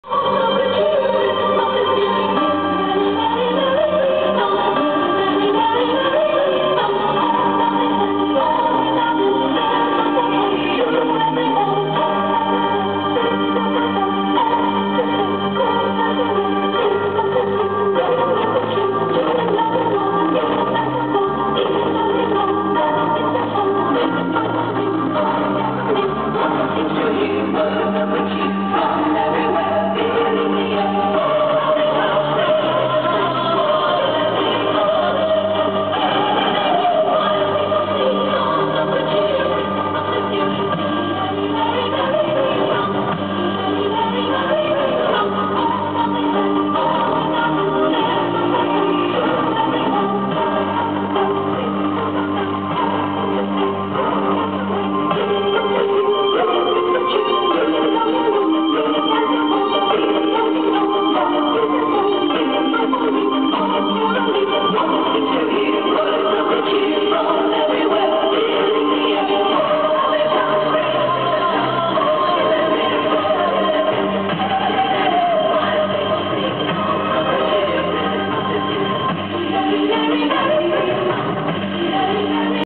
Island of Lights 2008-2009 Portage la Prairie, Manitoba
This was the Manitoba Hydro Power Smart 2010 Winter Games addition for the Portage la Prairie, Manitoba, Island of Lights during its 10th year anniversary.  Everyone loved this display that was syncronized to music.
It is an awesome display!  Watch for Island of Lights to re-open again towards the end of November (annually)!